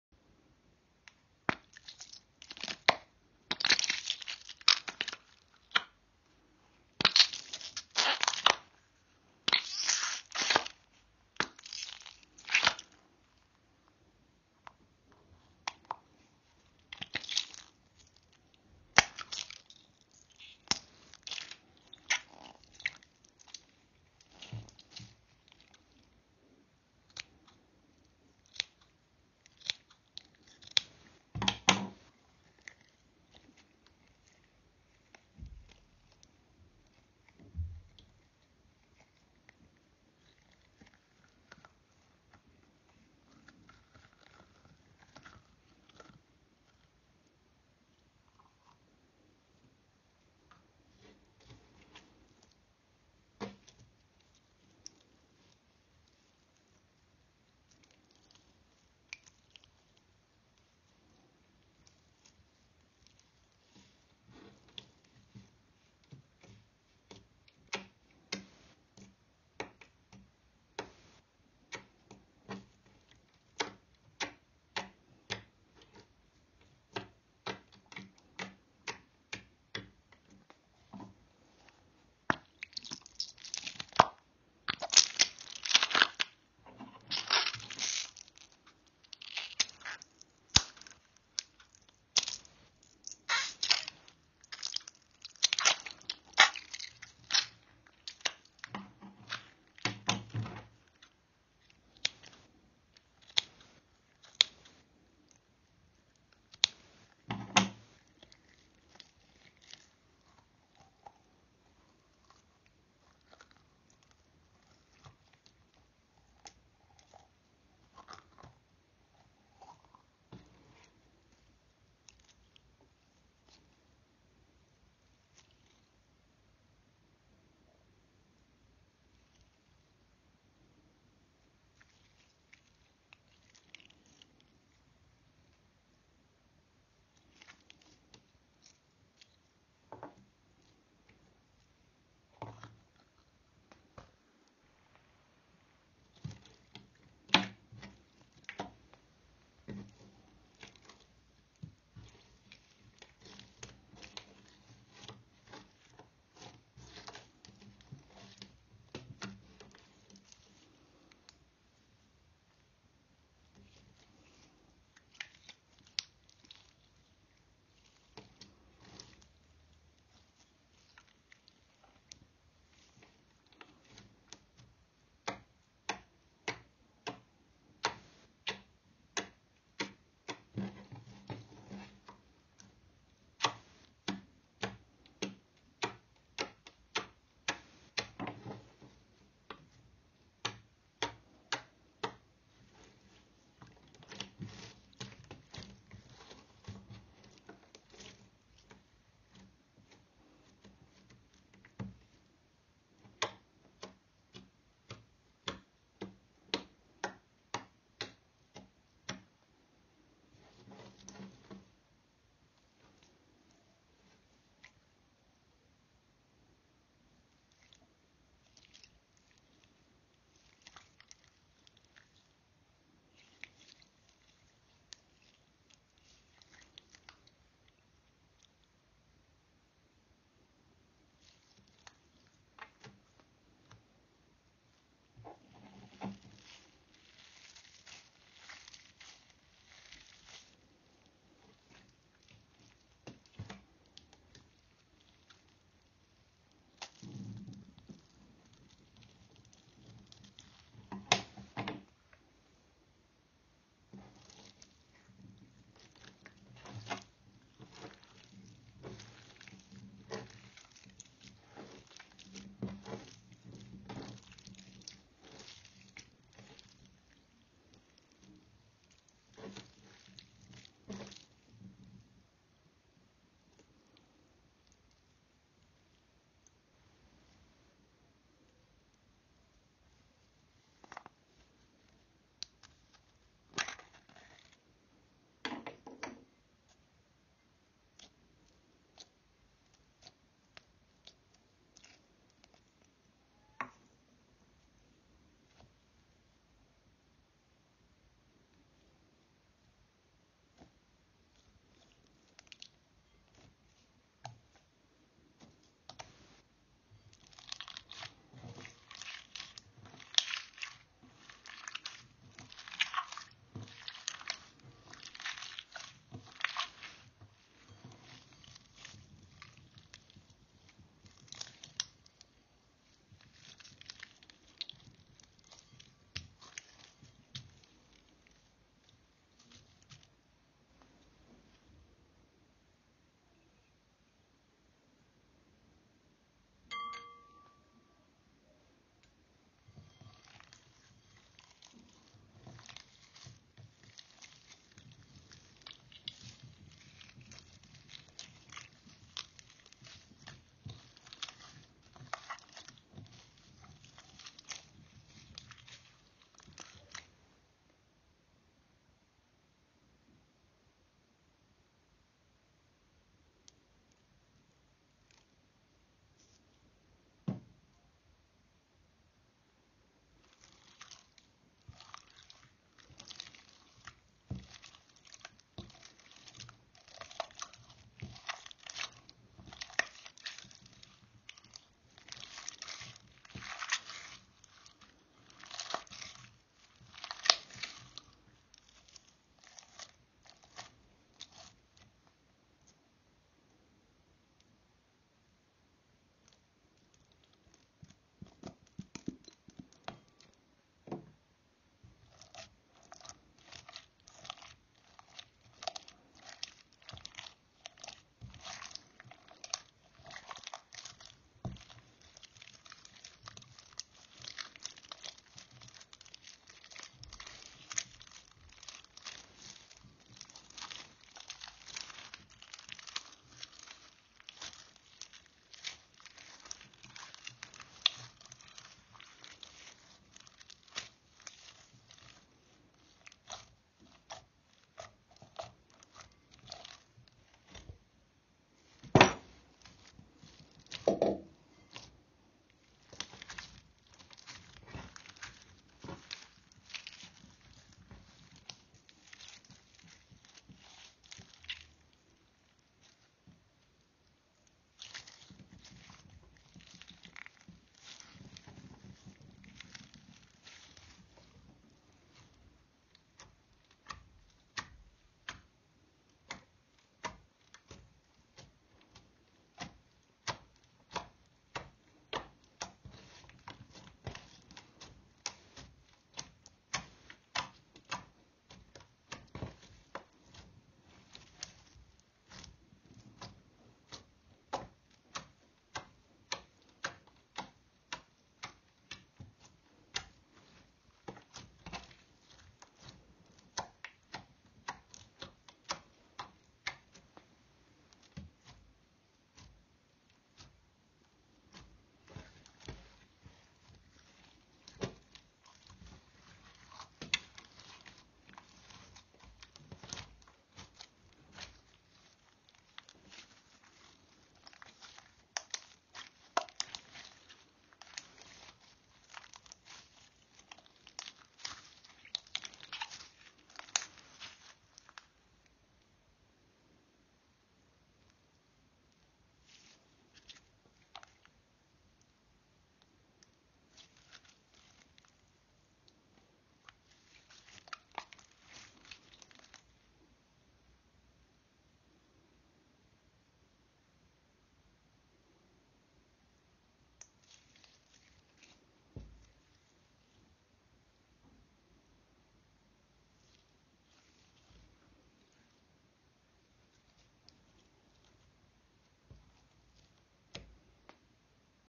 Mixing random things into store bought slime/slime satisfying /обзор слайма
Распаковываем антистрессовый лизун, и делаем слайм. Спасибо что смотрите моё видео.